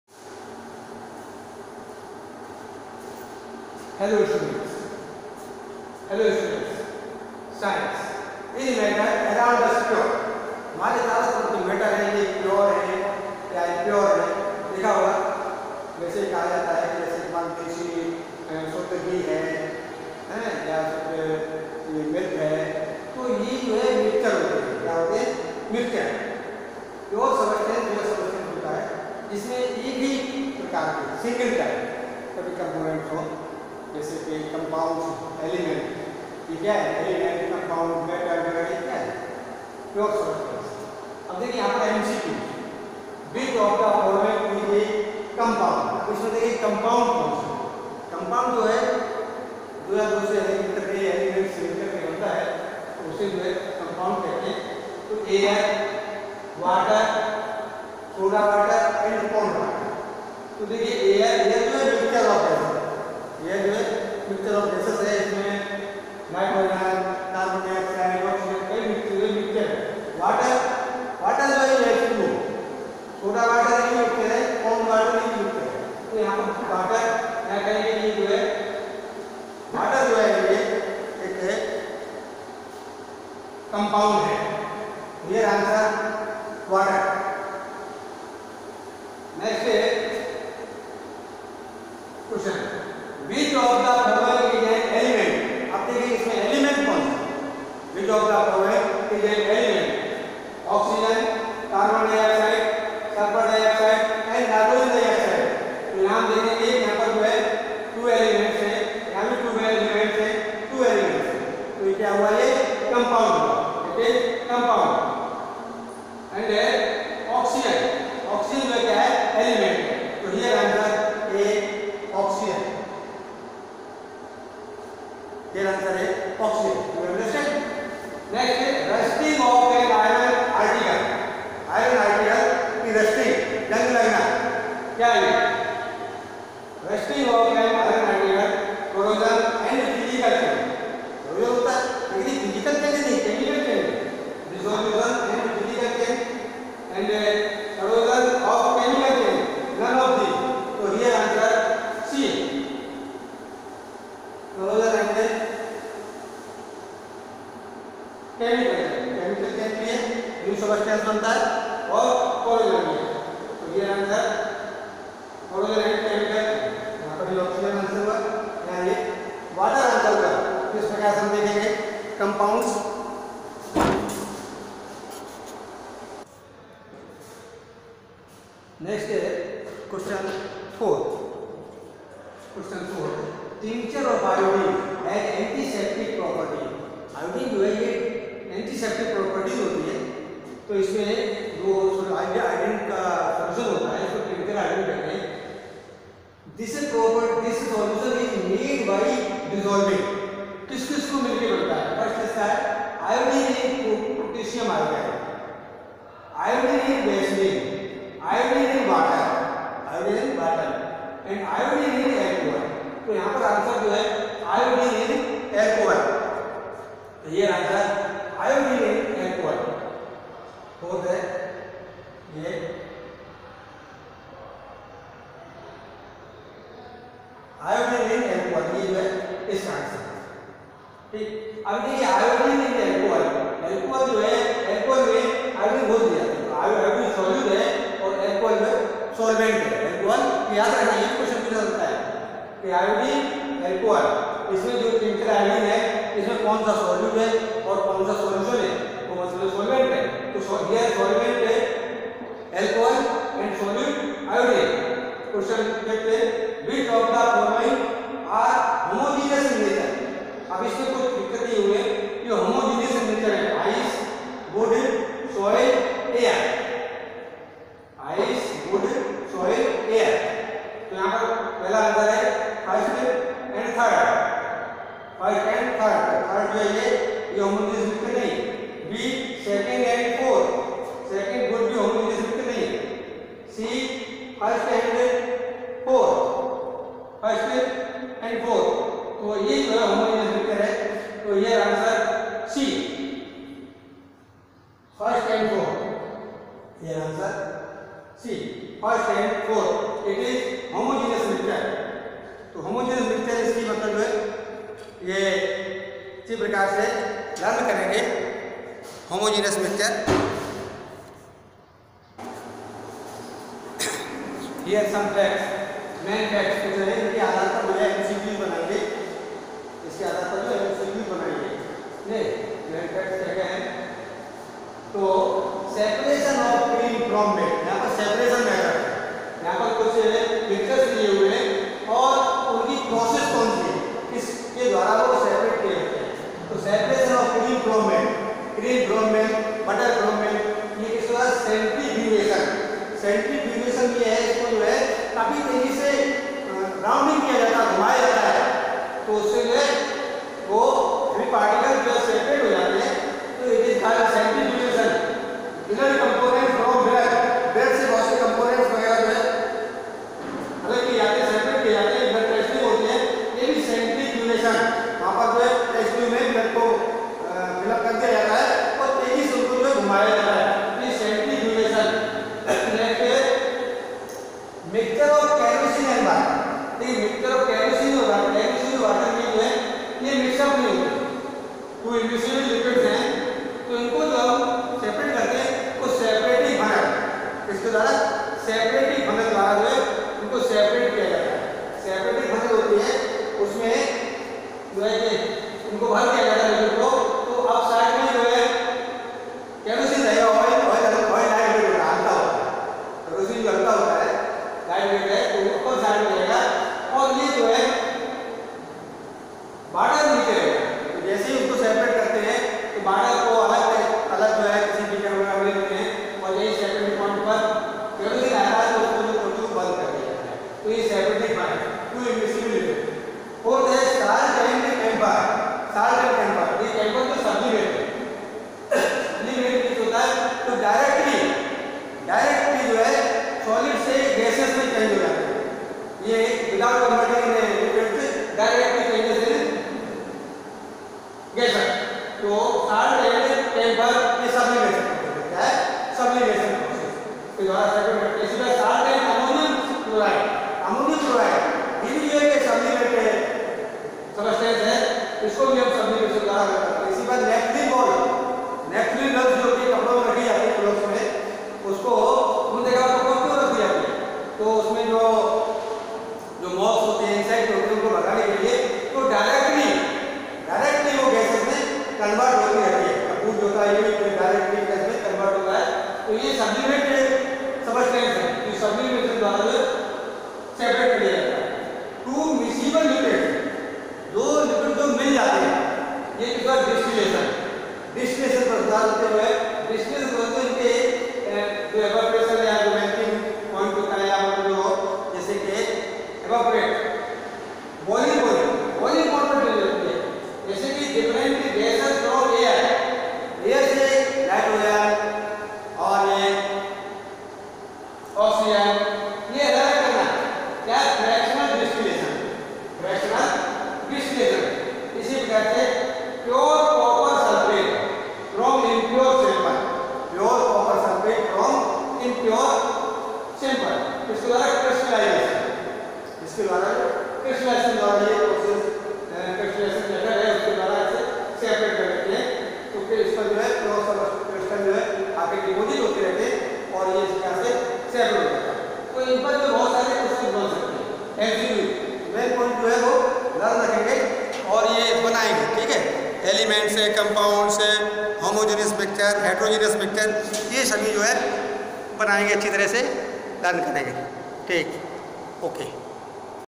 Class 9 , science chapter 2 ,ncert, is matter around us pure, multiple choice questions.
Class 9 , science chapter 2 , ncert  , is matter around us pure, multiple choice questions, be intelligent, be happy.